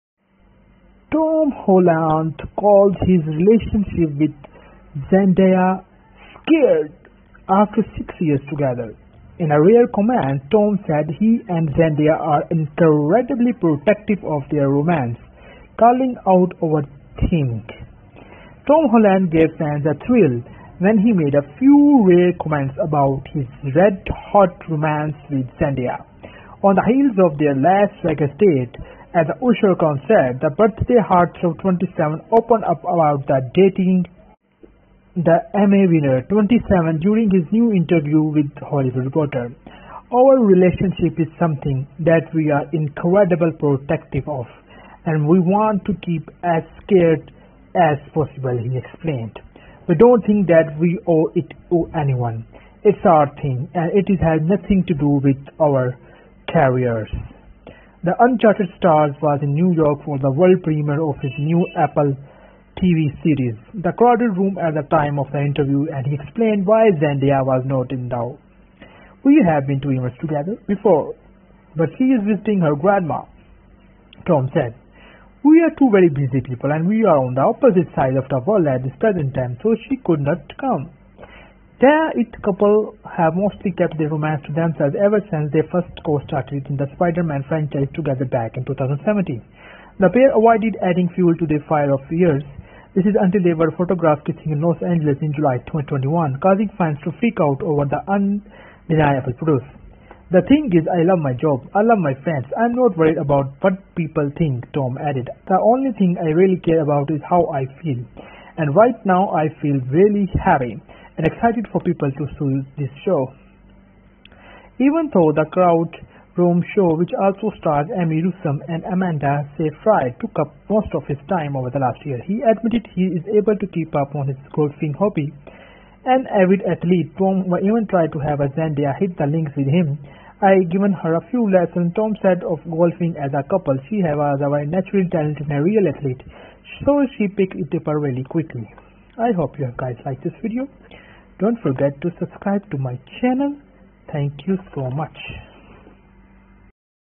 Tom Holland Calls His Relationship With Zendaya ‘Sacred’ After 6 Years Together
Hello Champions
Welcome to the most entertaining and informative Channel